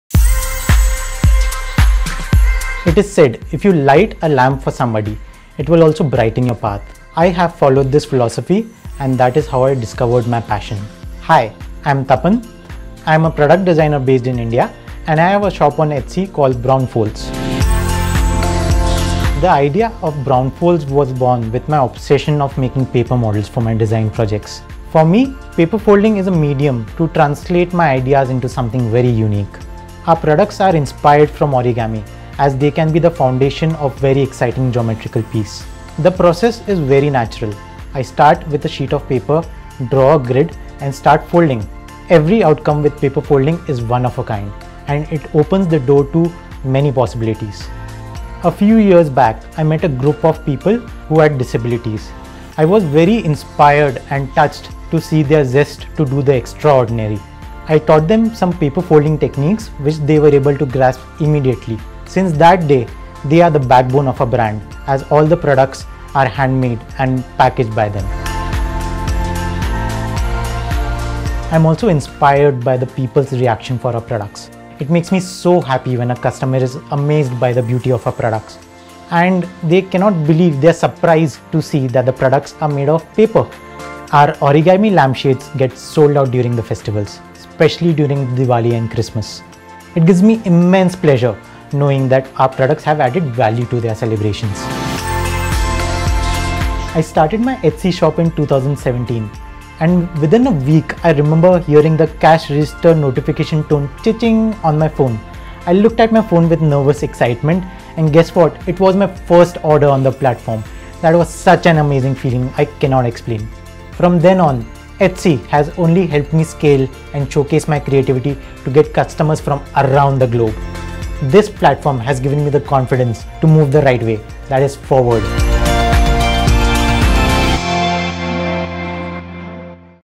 Meet Brownfolds
Etsy shop Brownfolds is the perfect combination of all things bright and beautiful! 🏮✨